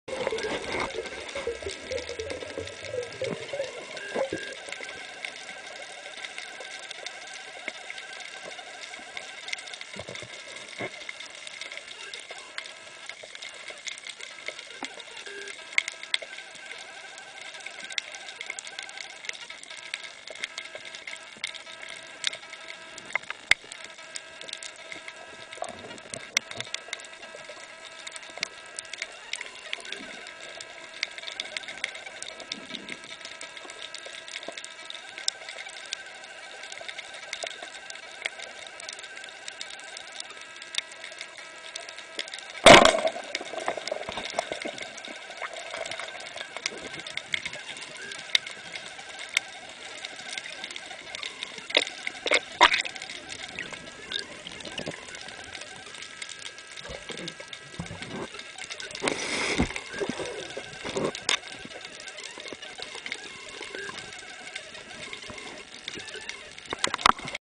zebra.MPG
Spearfishing a Zebra
Viz 4-5 m depth 12 m at Noordhoek